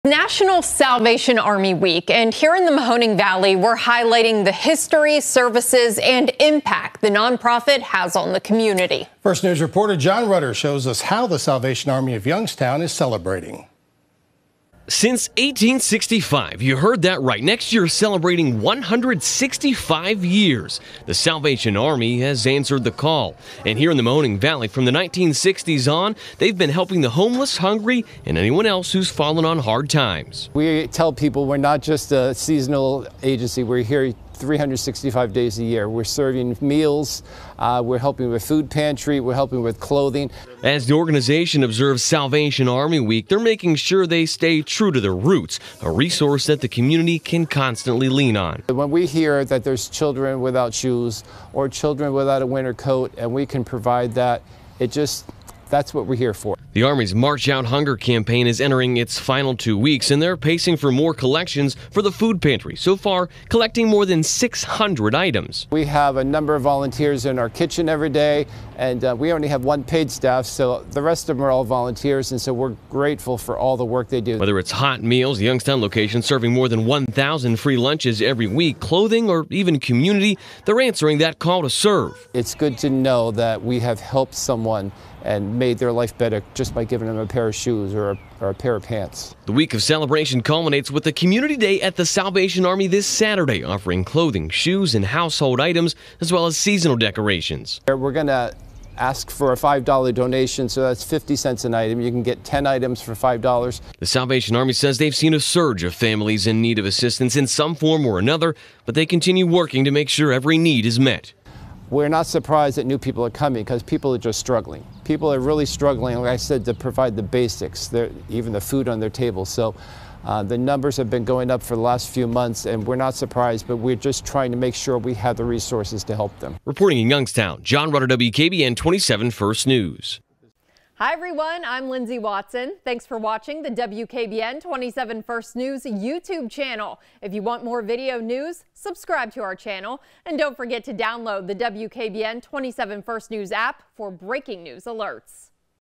Local organization celebrates National Salvation Army Week
Youngstown celebrates National Salvation Army week at the Salvation Army of Mahoning County